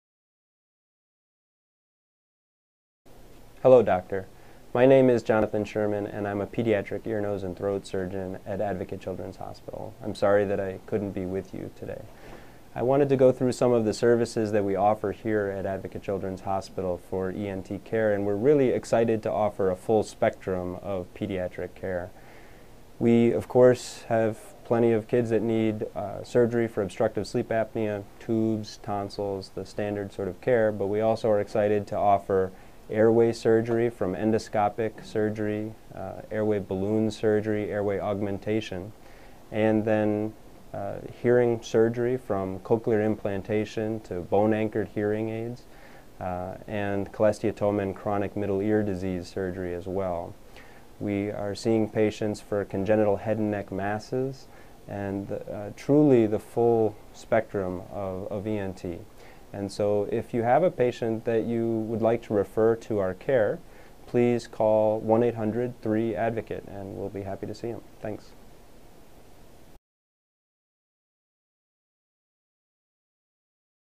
ENTCare Services at Advocate Children's Hospital
Dr. Jonathan Sherman, pediatric otolaryngology surgeon explains the range of surgical procedures offered at Advocate Children's Hospital.